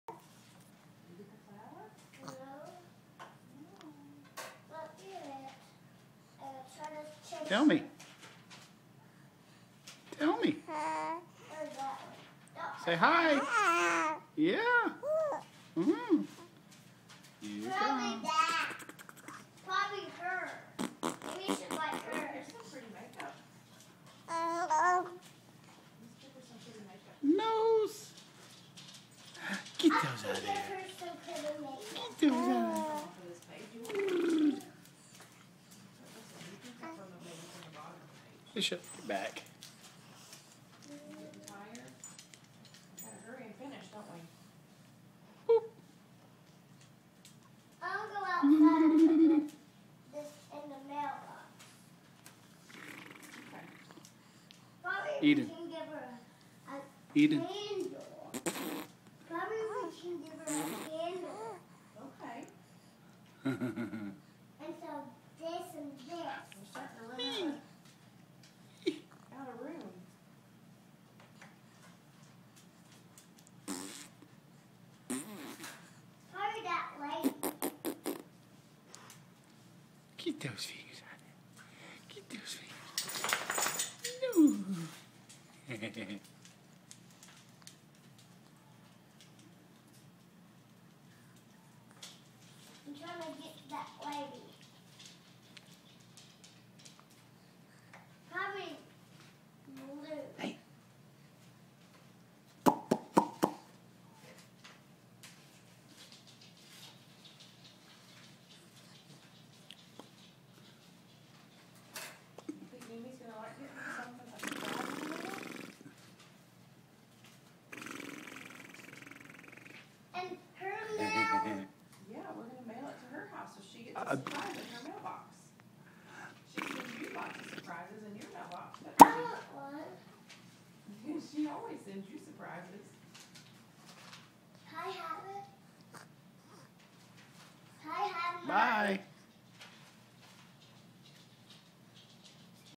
Beginning to babble
Eden is starting to babble a bit